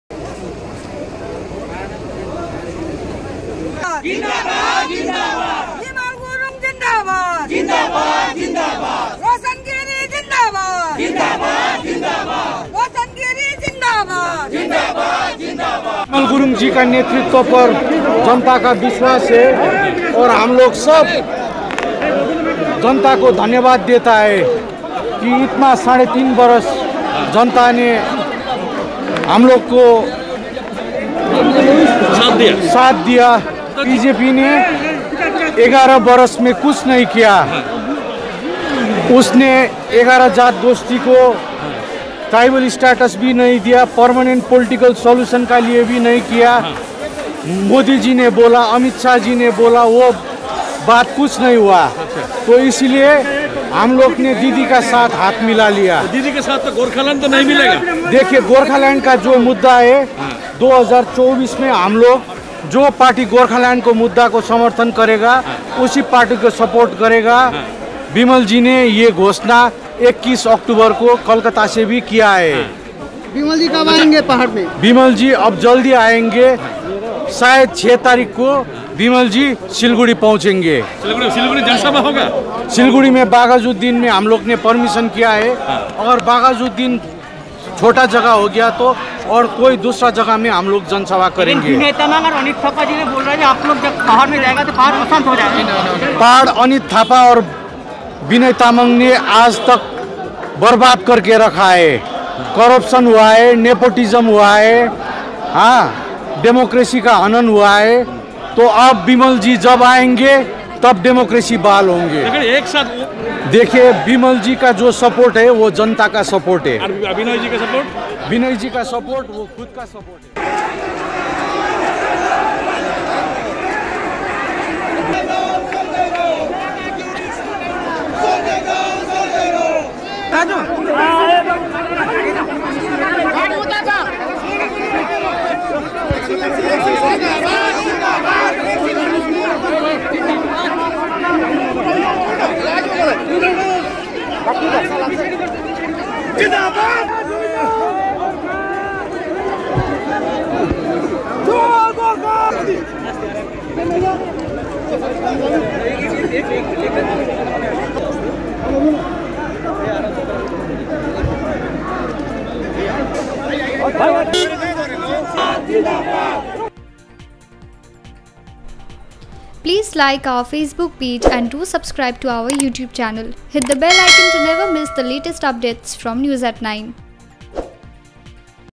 Morcha leader Roshan Giri to hold a rally in the hills after three years.
Morcha leader Roshan Giri finally arrived at Bagdogra airport today after three years. This happened after Morcha Chief Bimal Gurung went into an alliance with the Trinamool Congress just days ago. CM mamata Banerjee is not leaving any stones unturned prior to the elections in 2021.